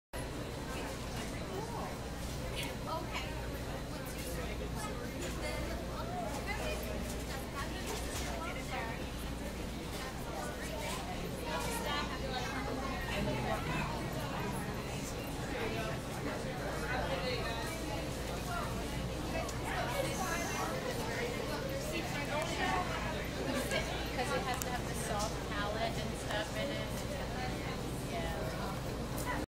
Breastfeeding Protest at Target store
women nationwide get together to breastfeed their children in Target to protest against a Texas mother being asked to leave Target after breastfeeding there.